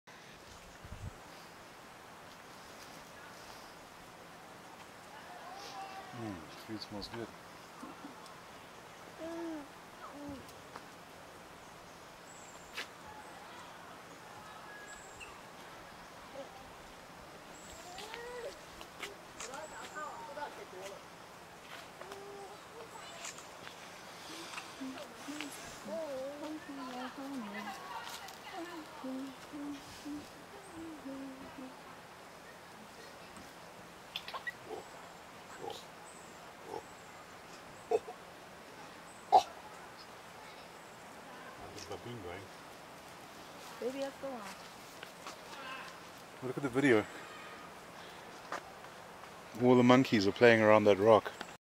Taiwan Nature: Monkeys having fun at Wuling farm, Taiwan
Monkeys having a good feed in the mountains, Wuling Farm, Taiwan.